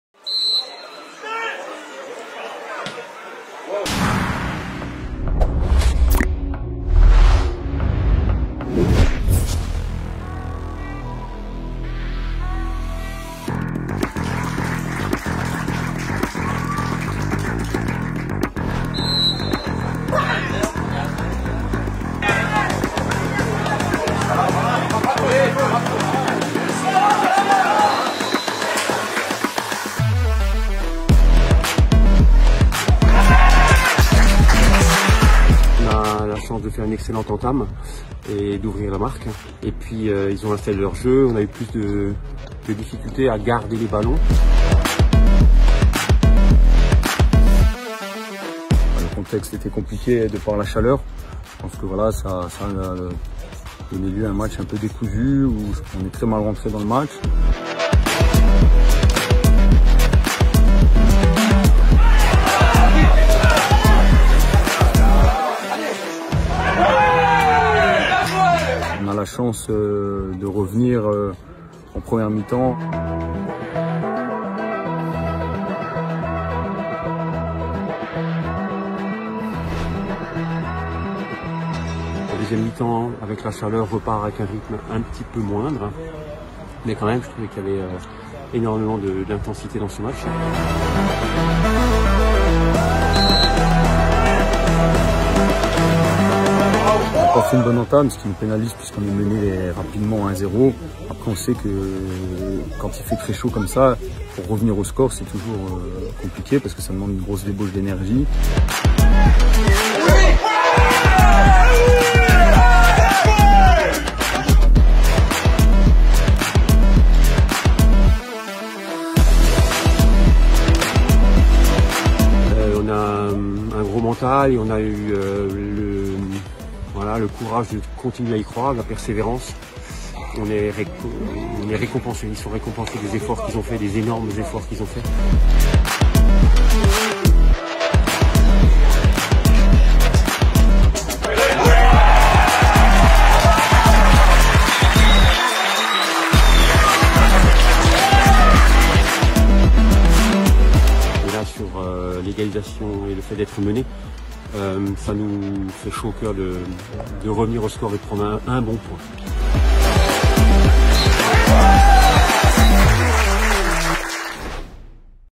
R1 Homiris SS Weyersheim - US Forbach (2-2) le résumé
Pas de gagnant aujourd'hui dans ce choc de régional où la Société Sportive de Weyersheim et l'US Forbach Football se neutralisent sur un score de 2-2. Un match rythmé et intense malgré la chaleur qui a mis les joueurs à l'épreuve!